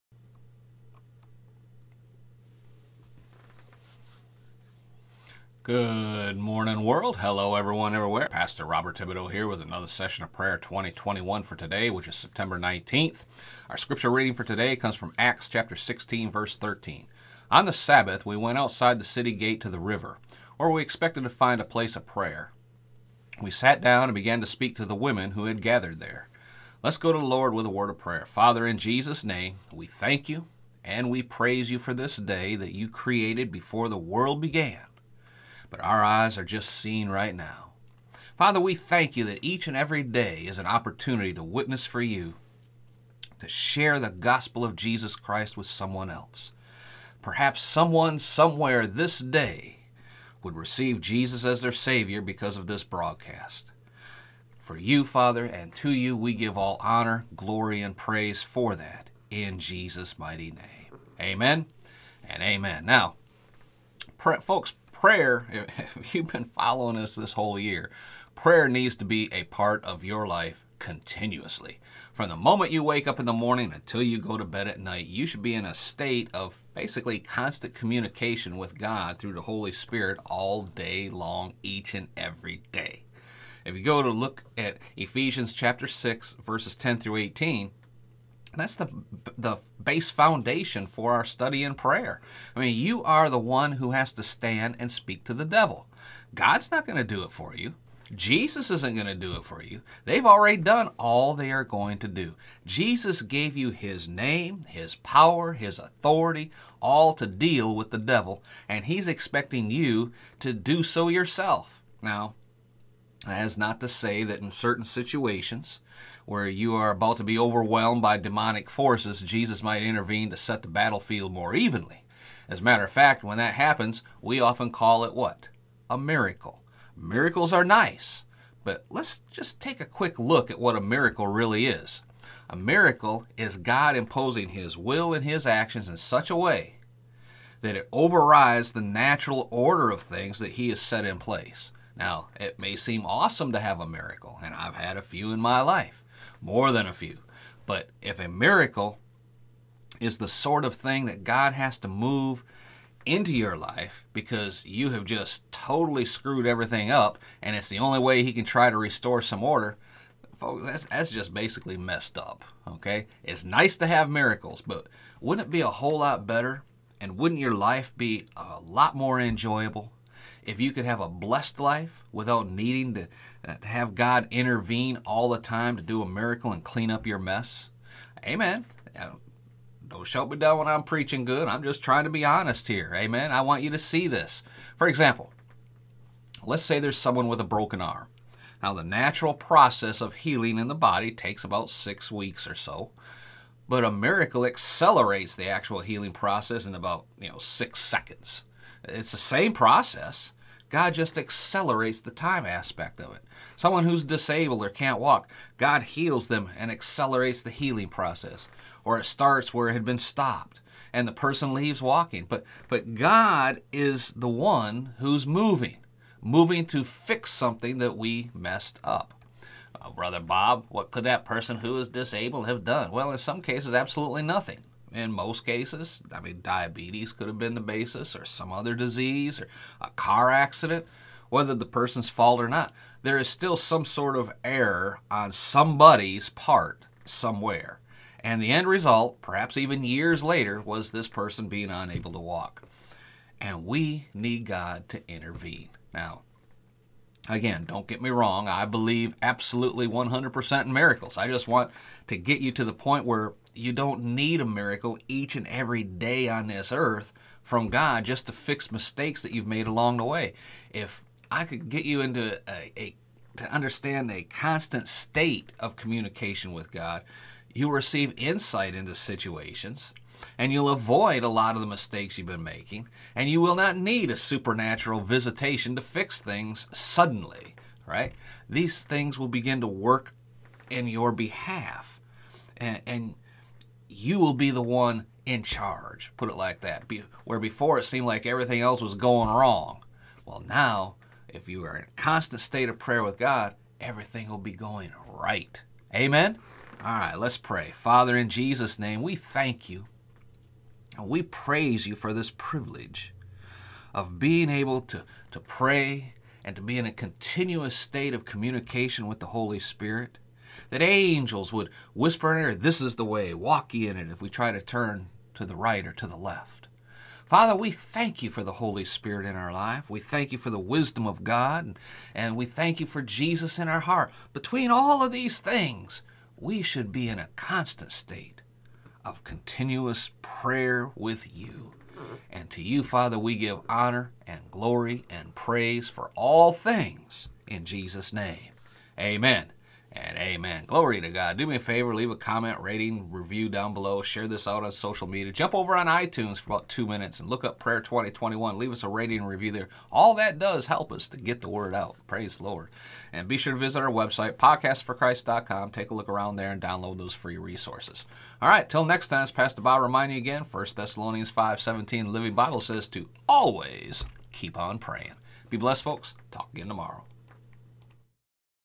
Prayer 2021 - September 19 - Prayer and Miracles pt 1
Scripture For Today:    Acts 16:13

“On the Sabbath we went outside the city gate to the river, where we expected to find a place of prayer. We sat down and began to speak to the women who had gathered there.”
Prayer and Miracles pt 1

Prayer ought to be part of your life – continuously.  From the moment you wake up in the morning until you go to bed at night, you should be in a state of constant communication with God – all day long each and every day!

Go to Ephesians 6:10-18 which is the base foundation for our study in prayer…

You are the one who has to stand and speak to the devil…God is not going to do it for you, Jesus is not going to do it for you…they have already done all they are going to do…Jesus gave you His Name, His Power, His Authority to deal with the devil and He is expecting you to do so yourself.  Now, that is not to say, in certain situations, where you are about to be overwhelmed by demonic forces – Jesus might intervene to set the battlefield more evenly….as matter of fact, when that happens – we call it a “miracle.”  Miracles are nice…but, let’s take a quick look at what a miracle really is.

A miracle is God imposing His Will and Actions in such a way as to override the natural order of things….now, it may be awesome to have a miracle…and I have had a few in my life…more than a few…but, if a miracle is the sort of thing that God has to have move into your life because you have just totally screwed everything up and it is the only way He can try to restore some order…that’s “messed up.”  It is nice to have miracles…but wouldn’t it be a whole lot better and wouldn’t your life be more enjoyable…if you could live a Blessed Life without needing God to intervene all the time to do a miracle and clean up your messes? Amen!  I’m not trying to be mean here, just honest…I want you see this.

For example… Let’s say there is someone with a broken arm…the natural process of healing takes about 6 weeks or so….but, a miracle accelerates the actual healing process into about 6 seconds!!!  Same process – God just accelerates the time aspect!  Someone who is disabled, can’t walk…God heals them and accelerates the healing process – or starts it where it had stopped – and the person leaves walking….but God is the one who is moving… moving to FIX something that WE have messed up..

“Brother Bob, what could that person who is disabled could have done….?”  Well, in some cases – absolutely nothing…but in most cases…diabetes could have the basis…or some other disease…car accident, whether the persons fault or not…there as still some sort of “error” on “somebody’s” part…  that the end result, perhaps even years later, was this person being unable to walk.  And we need God to intervene.

Now, again, don’t get me wrong…I believe in miracles….I just want to get you to the point where you don’t need a miracle each and every day from God, just to fix mistakes you have made…..if I can get  you into a constant state of communication with God, you will receive insight into situations and you will avoid a lot of the mistakes you have been making…and you will not need a supernatural visitation to fix things…suddenly – things will begin to work in your behalf, where before it seemed like everything was going wrong….amen!!!

Let’s Pray!